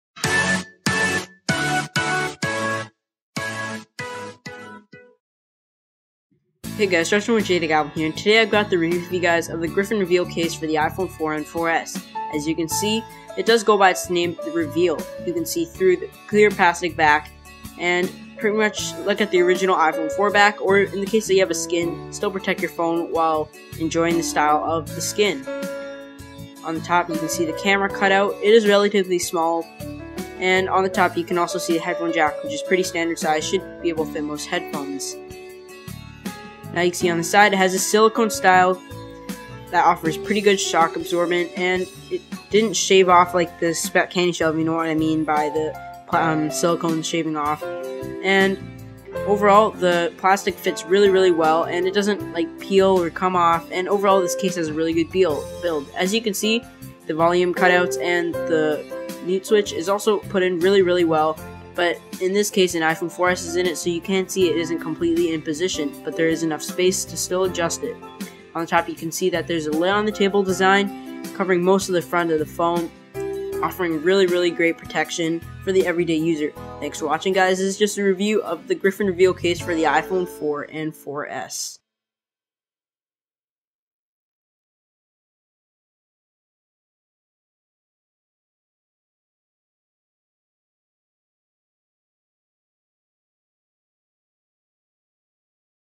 Griffin Reveal iPhone 4/4S Case Review
Review of the Griffin Reveal case for the iPhone 4/4S

Product Link: goo.gl/cEUkX